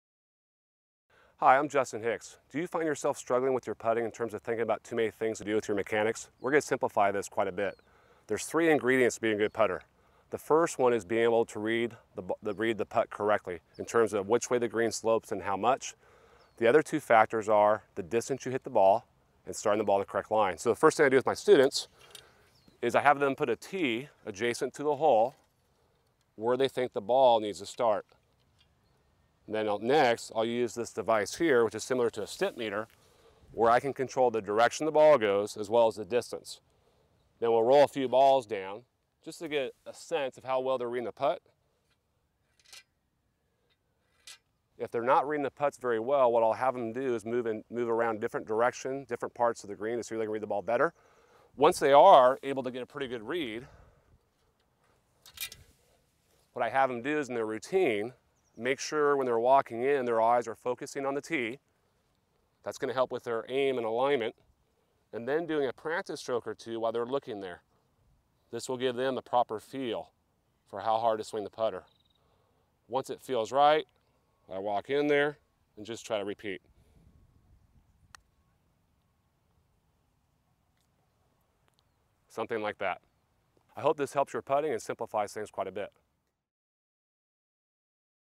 Golf Putting Tips: How to Improve your Golf Putting Technique - National University Golf Academy
today to learn more about training for a career in golf at National University Golf Academy.

Justin Hicks, Golf Instructor at Stadium Golf Center, explains how to improve your golf putting technique with a simple golf drill. We look forward to your feedback.

Hi I'm Justin Hicks. Do you find yourself struggling with your putting in terms of thinking about too many things to do with your mechanics? We're going to simplify things quite a bit. There are 3 ingredients to being a good putter.

1. The first one is being about to read the putt correctly in terms of which way the green slopes and how much.
2. The other two factors are the distance you hit the ball and ...
3. Starting the ball on the correct line.

The first thing I do with my students is I have them put a tee adjacent to the hole where they think the ball needs to start. Then next I'll use this device here, which is similar to a stint meter, where I can control the direction the ball goes as well as the distance.  Now we'll role a few balls down, just to get a sense of how well they are reading the putt. If they are not reading the putts very well what I'll have them do is have them move around different directions, different parts of the green so the can read the ball better.

Once they are able to get a pretty good read what I have them do in their routine is make sure when they're walking in, their eyes are focusing on the tee. That's going to help with their aim and their alignment and then doing a practice stroke or two while they're looking there.  This will give them the proper feel for how hard to swing the putter.  Once it feels right, I walk in there and just try to repeat. Something like that.

I hope this helps your putting and simplifies things quite a bit.